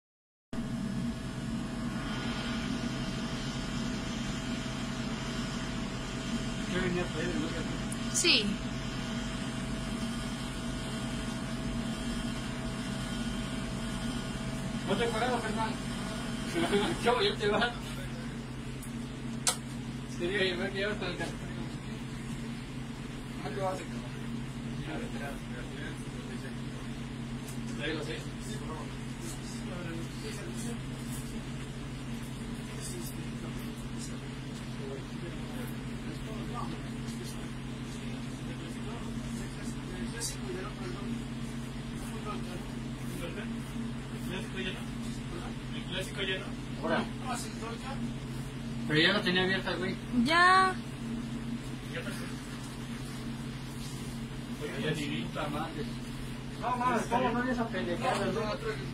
Sujeto agrede a empleada por no venderle cerveza en Puebla
Video de: Emm Herrera
Fue captado el momento en que una mujer que estaba atendiendo una tienda en la capital poblana, fue agredida por un cliente que exigía que le vendiera bebidas alcohólicas.

De acuerdo con la grabación, durante el fin de semana; un hombre ingresó a un establecimiento ubicado en el Fraccionamiento Los Héroes Puebla. Se observa que le exige que le venda una cerveza, sin embargo, la encargada le menciona que no puede comercializar ese producto debido a que el gobierno prohibió su venta durante el fin de semana.

En reacción, el hombre agrede a la mujer con una lata de cerveza y le avienta el líquido.
Información: telediario.mx